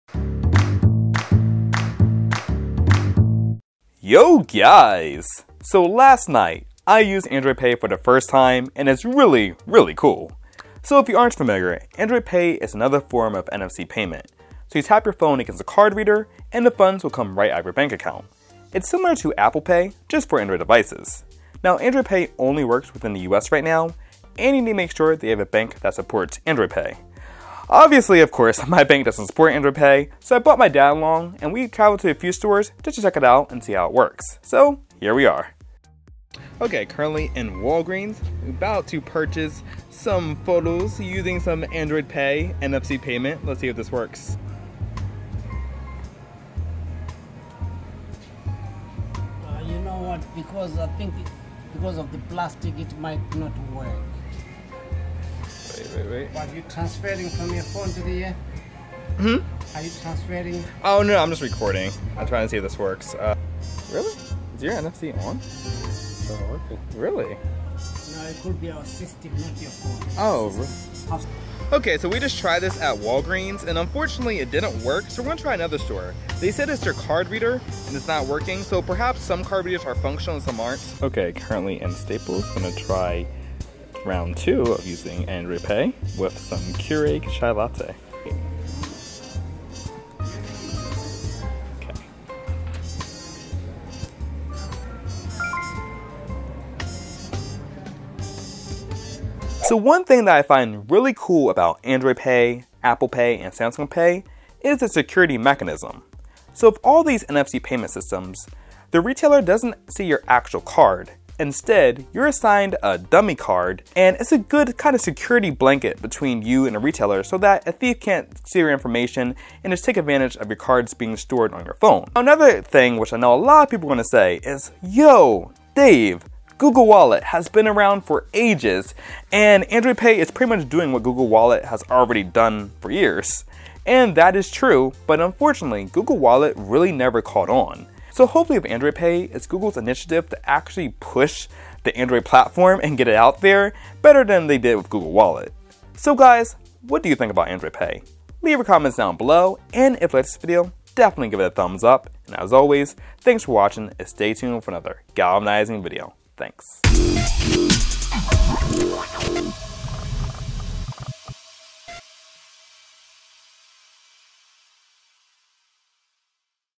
Using Android pay in Store
Spent the night visiting stores and using Android pay. Here's what happened.